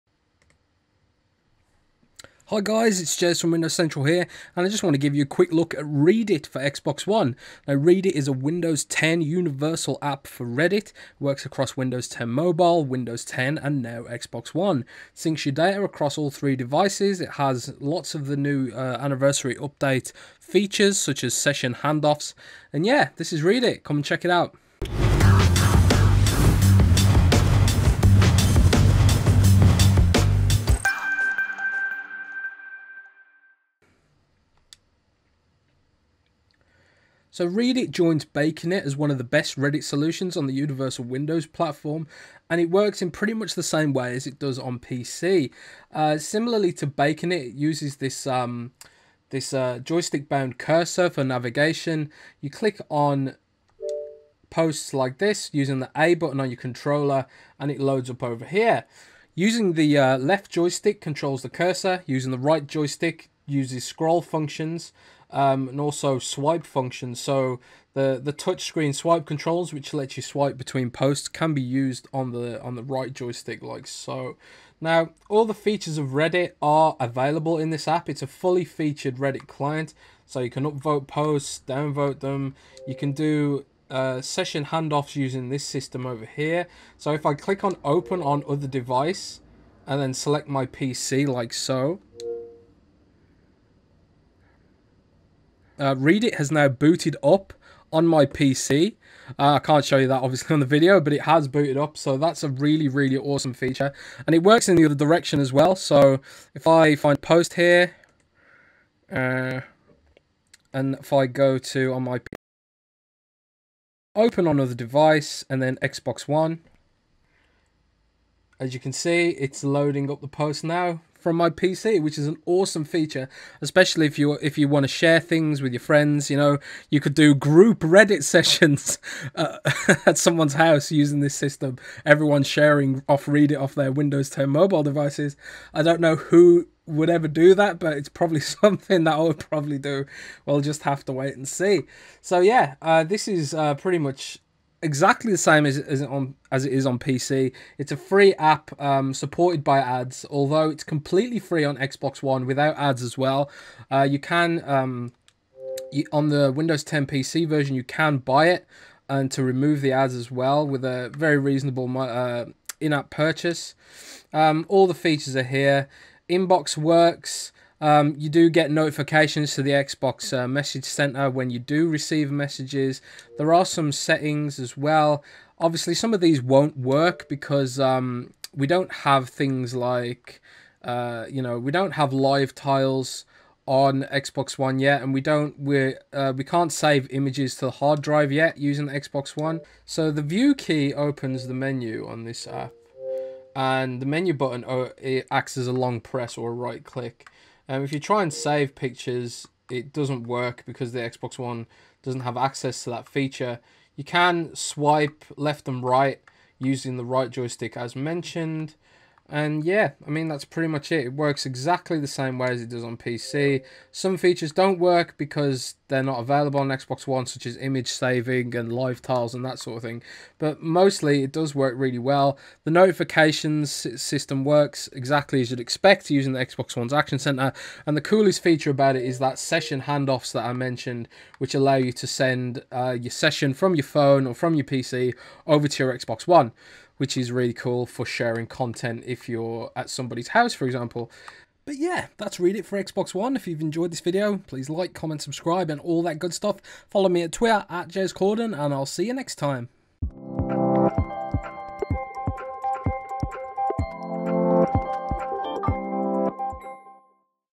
Hands-on: Readit (for Reddit) on Xbox One!
Readit has hit Xbox One with session hand-offs between Windows 10 PC and Mobile!